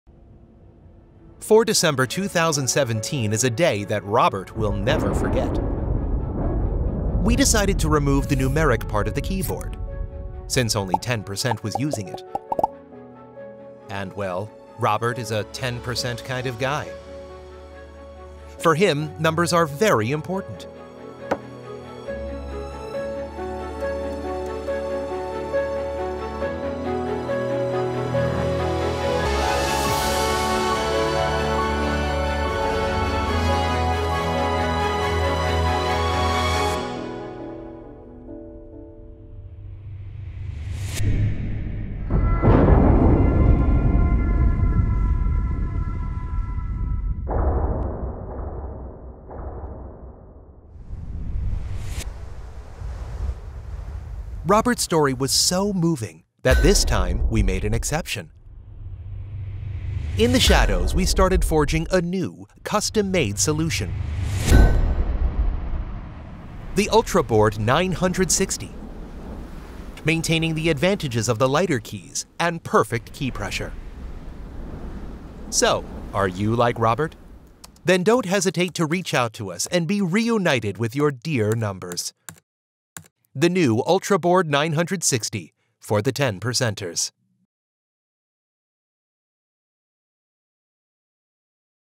BakkerElkhuizen | UltraBoard 960 Compact Standard | FR
Avec l'UltraBoard 950, nous avons décidé de supprimer la partie numérique du clavier, puisque seulement dix pour cent l'utilisaient.

Et bien .. il y a encore dix pour cent de gens là-bas. Pour eux, les chiffres sont très importants. Cette fois, nous avons fait une exception. Dans l'ombre, nous avons commencé à forger une nouvelle solution sur mesure.

Présentation du nouveau UltraBoard 960 Compact Standard. Maintien des avantages des touches plus légères et pression parfaite des touches.

Soyez réunis avec vos chers numéros.